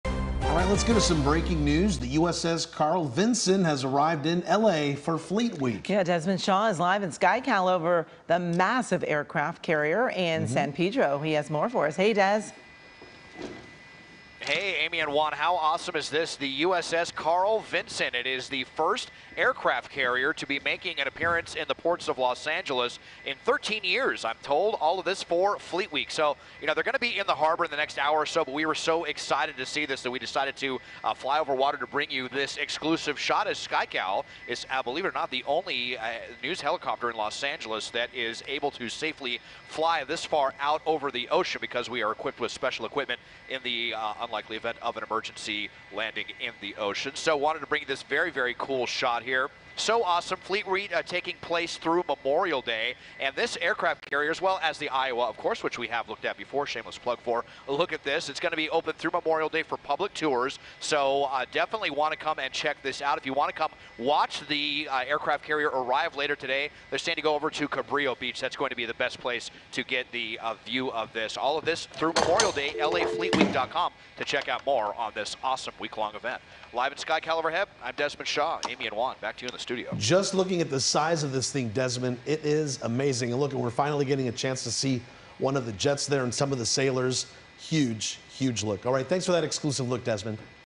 USS Carl Vinson arrives to LA for Fleet Week
The USS Carl Vinson is the first aircraft carrier in the last 13 years to appear in LA.